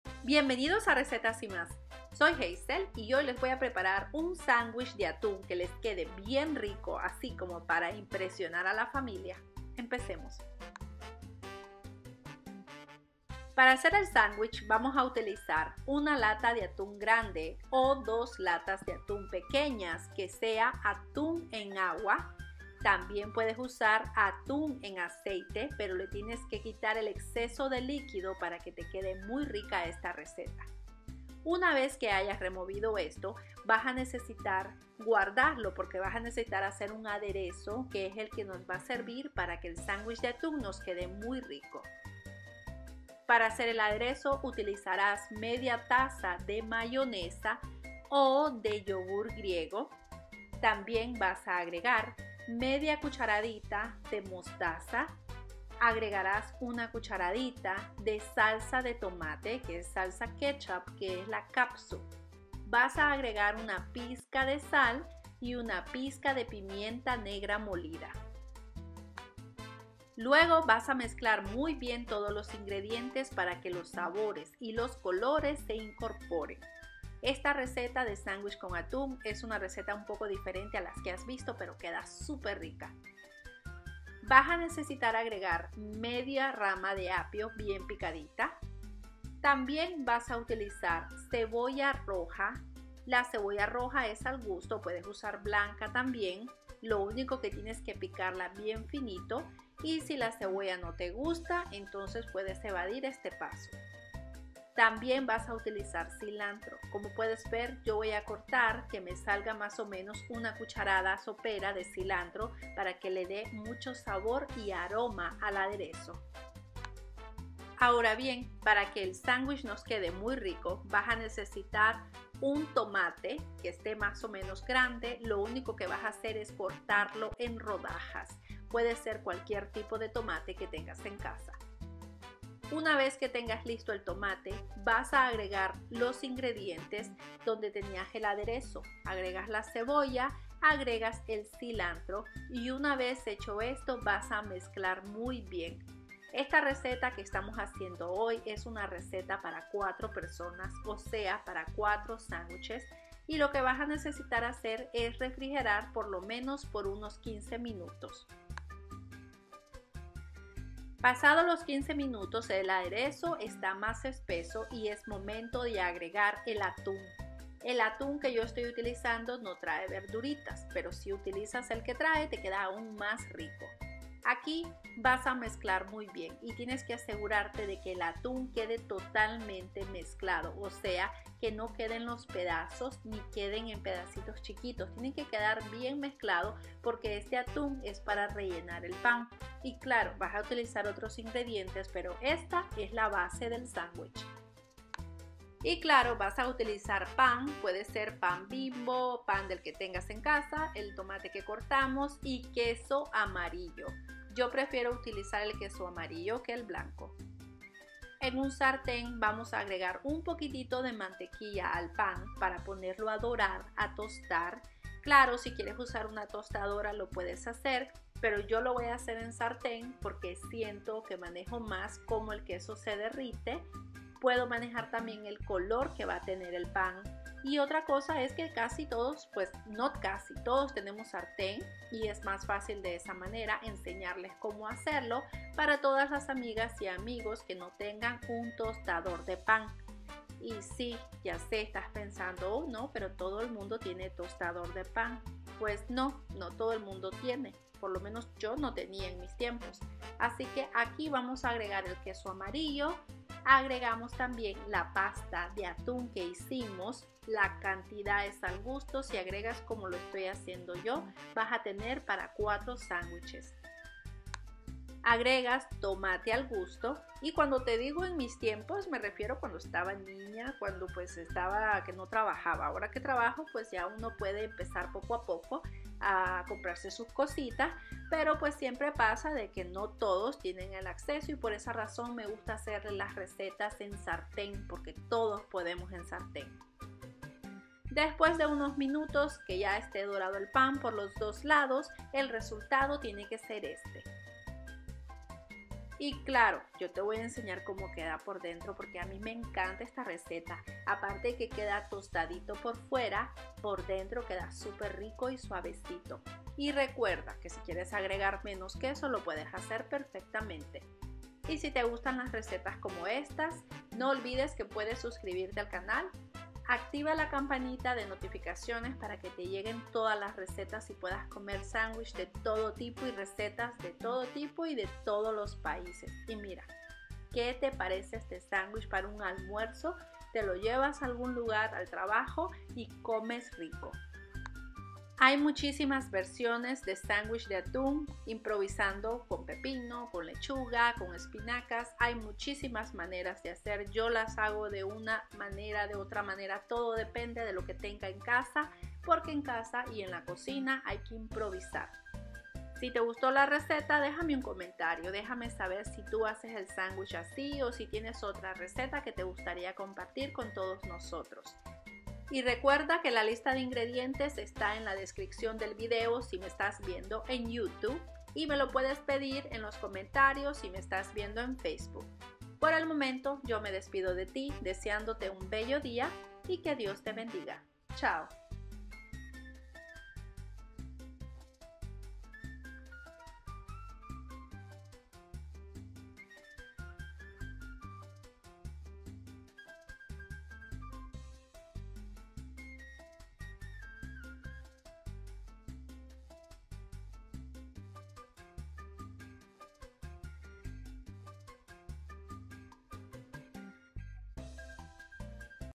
Sandwich de Atún bien sabroso, fácil y económico de hacer
Sandwich de atún, una receta perfecta para almorzar o cenar. Recetas rapidas con atun. Recetas faciles. Como hacer sandwich de atún. Aqui encontraras recetas faciles y rapidas de hacer para toda la familia.

Ingredientes
1 lata de atún grande o 2 pequeñas
1 tomate
queso amarillo
1/2 taza mayonesa
1/2 cdta mostaza
1 cdta salsa tomate
1/4 cebolla
1/2 rama de apio
pan de molde
pizca pimienta negra